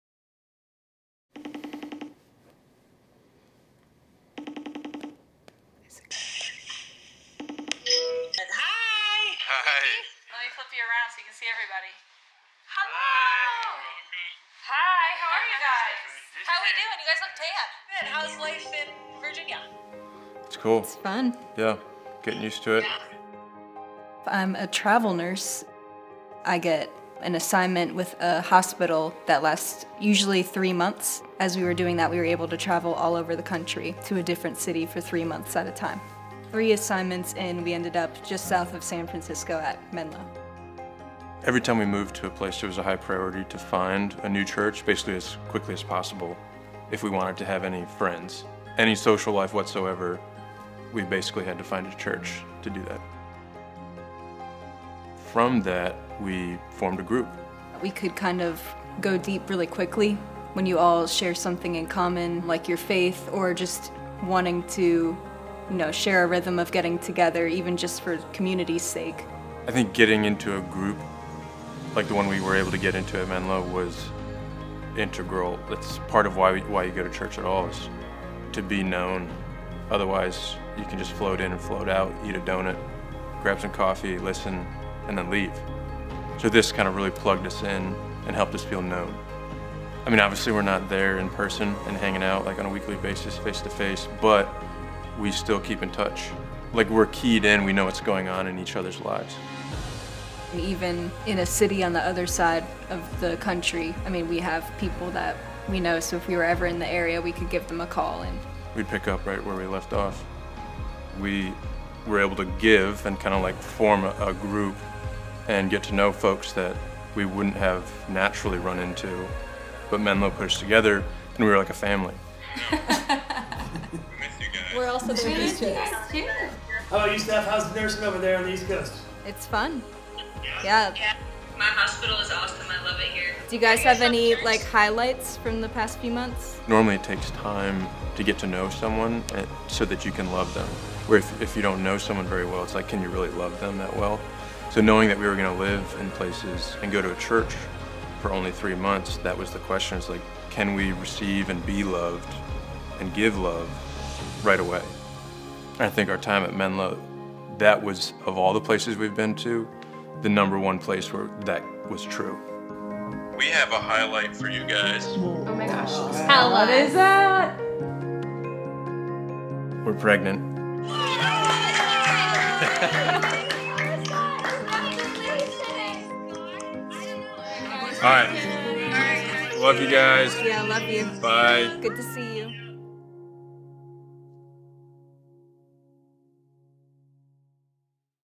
Is It Possible To Love Immediately? BELONG: Menlo Church "Life Group" Video
My wife and I told our story to John Ortberg's megachurch in Silicon Valley about how we knew we weren't going to be around for very long but still decided to jump into community and do our best to love people and receive love while we were there.